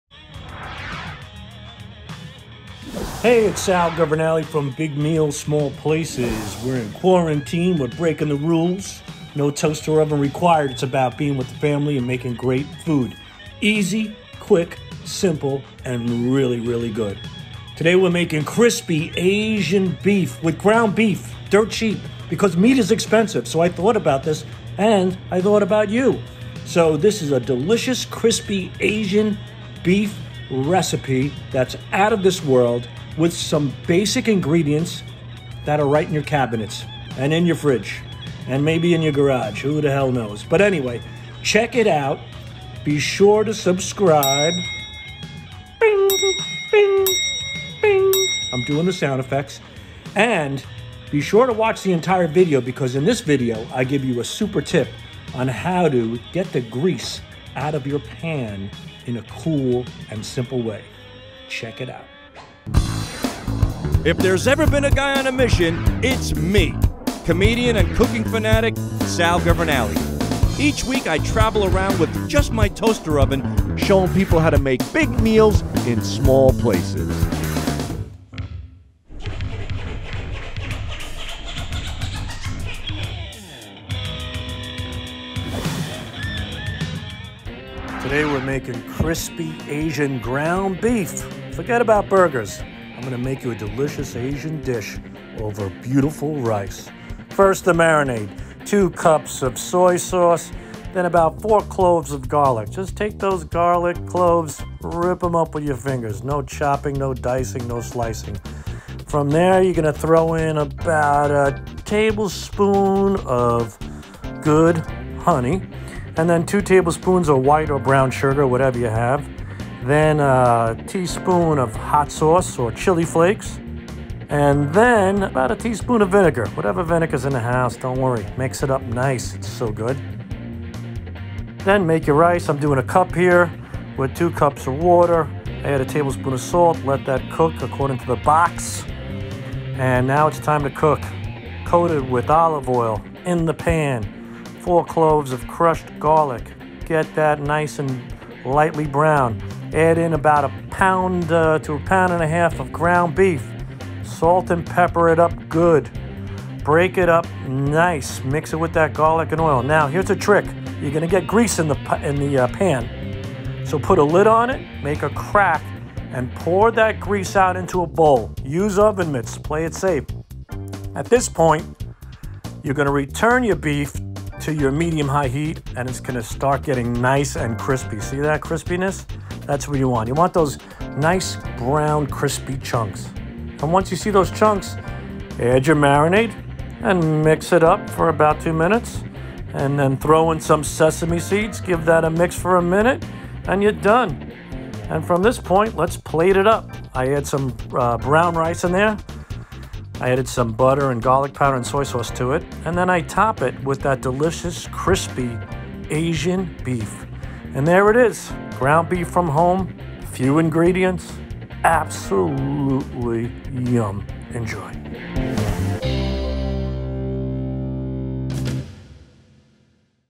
Easy Crispy Asian Beef with Sal Governale | QUICK RECIPE
This recipe is an easy and cheap way to make Korean ground beef over rice. Sal Governale shows you a quick way to cook this classic Asian meal.  Follow along as Sal makes the marinade, the crispy beef, and perfect rice in just a few short minutes.